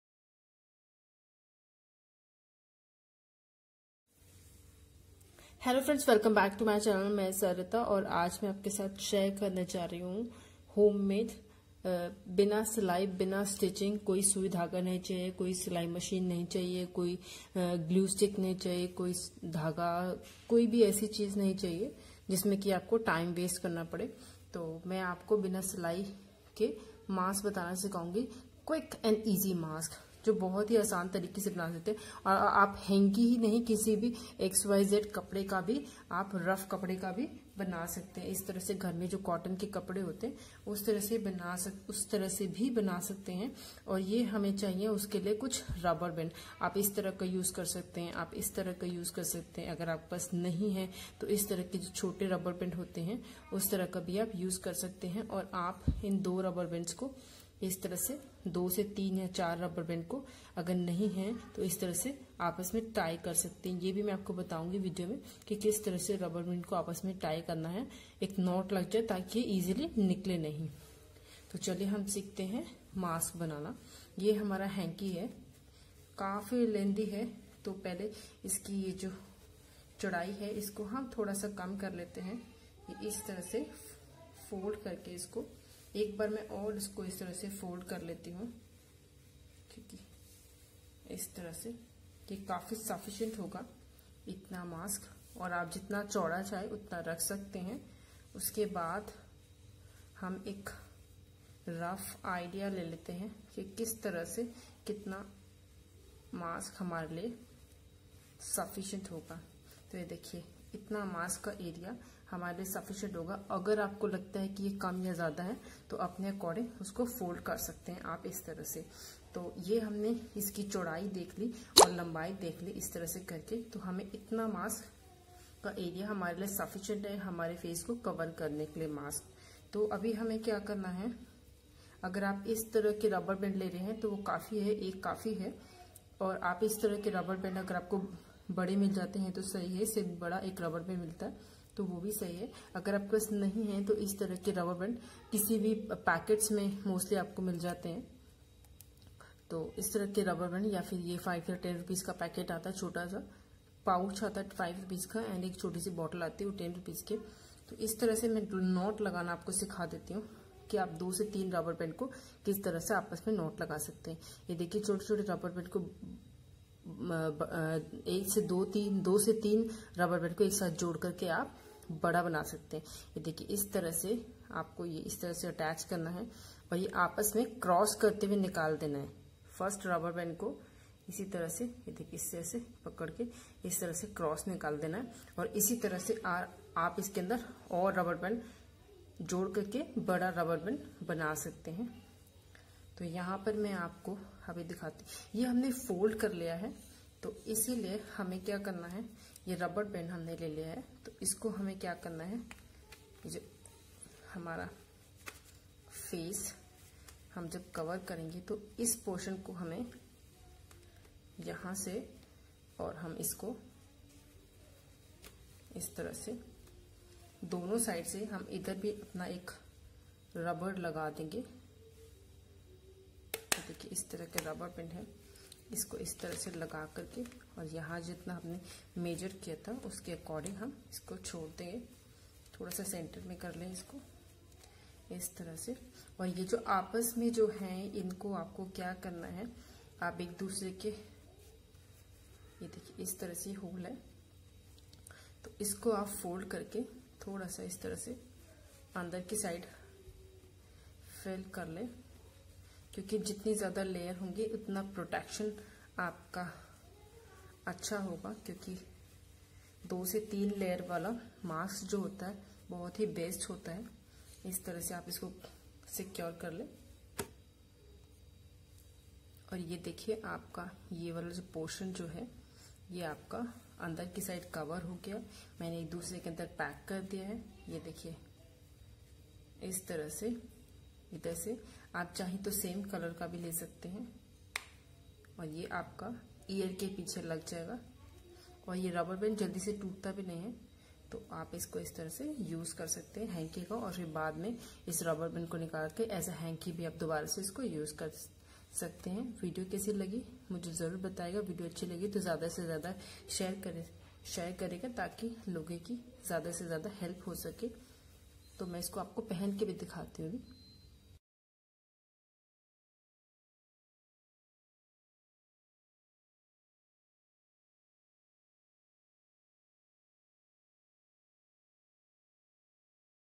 DIY NO SEW FaceMasks/ Face Mask Making at Home/Surgical Face Mask /Easy and Quick Mask Making Idea
DIY FaceMask/FaceMasks/Face Mask Making at Home/Surgical Face Mask at home/Easy and Quick Mask Making Idea,No SEW

Hi... I'm SARITA TONGAR in this video I'm sharing

Covid-19 At Home/3D#FaceShield.

If you like this video, plz leave your valuable comment.